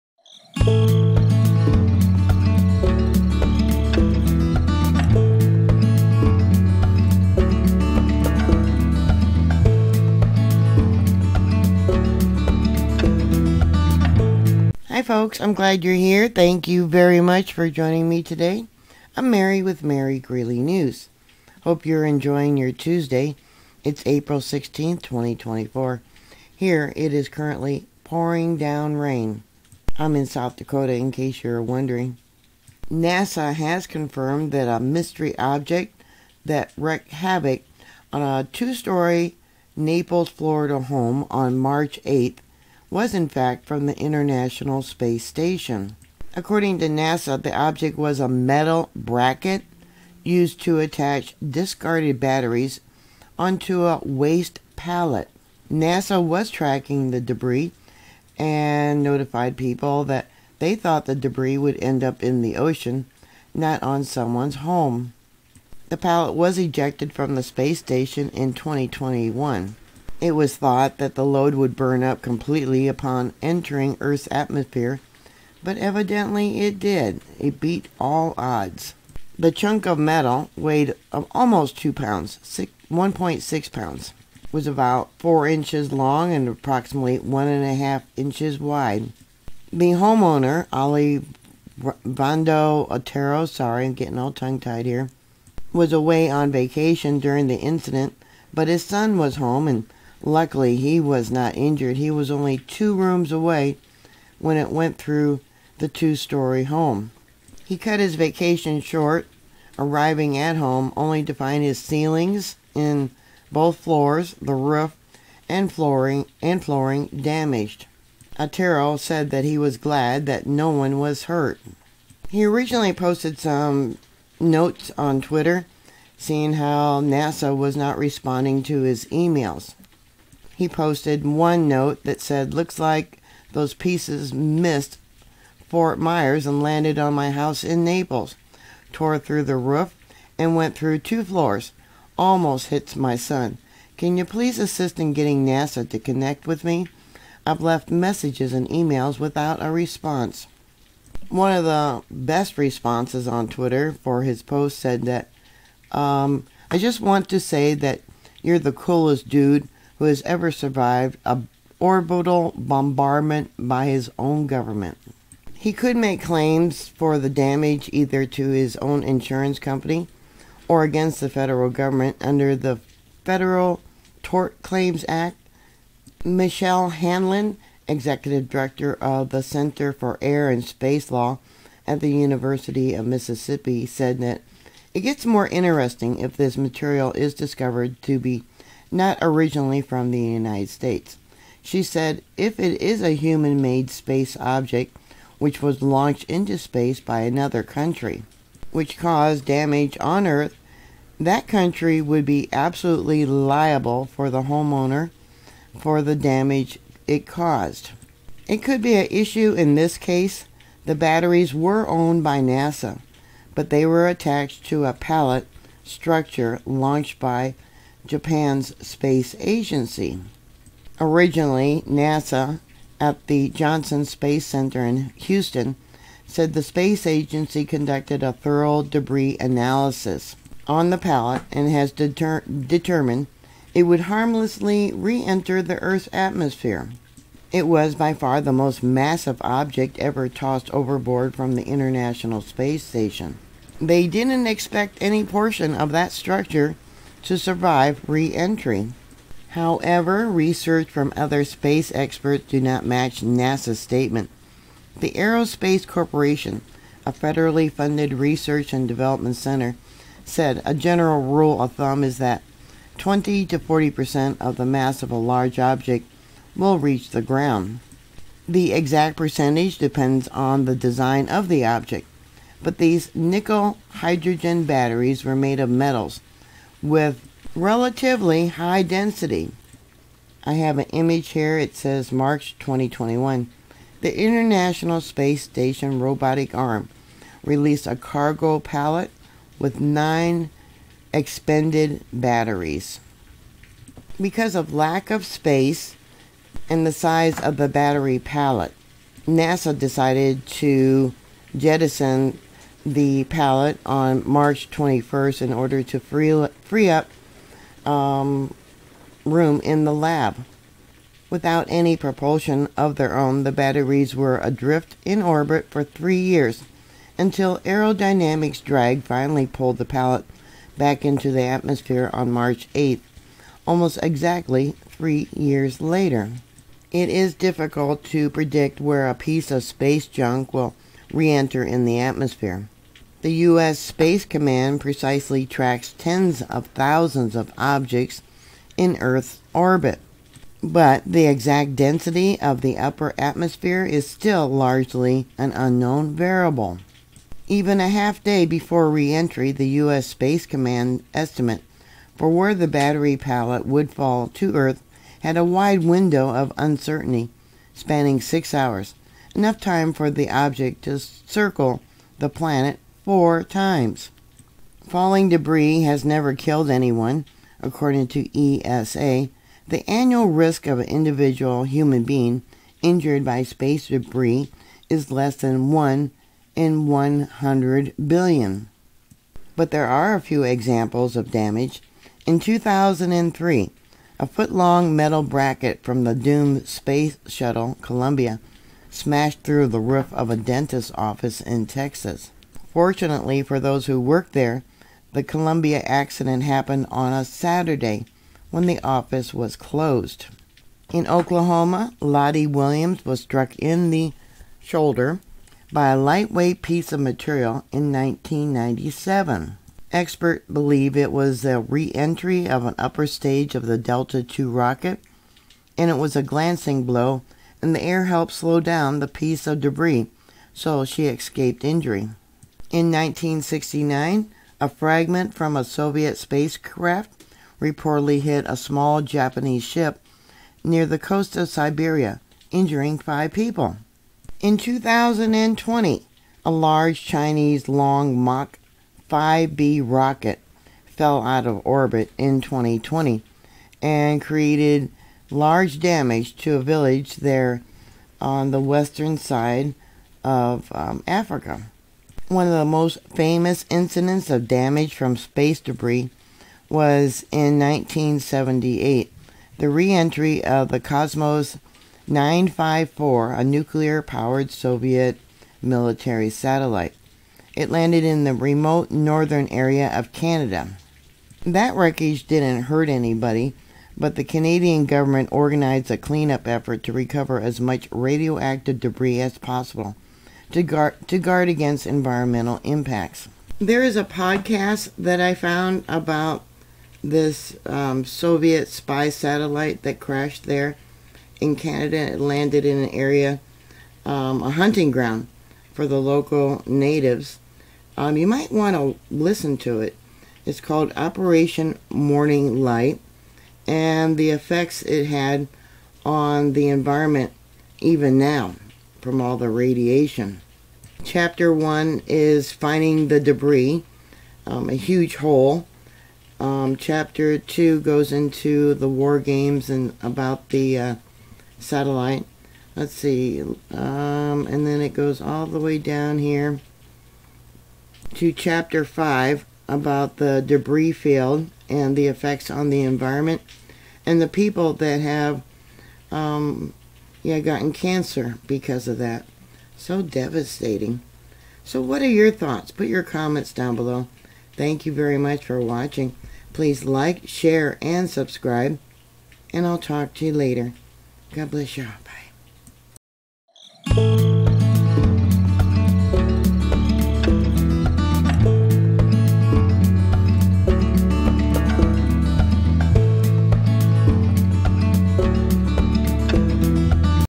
NASA Space Junk Smashes Through Florida Homeowner's Roof, Operation Morning Light
Podcast
1978 Canada / Russia Satellite Crash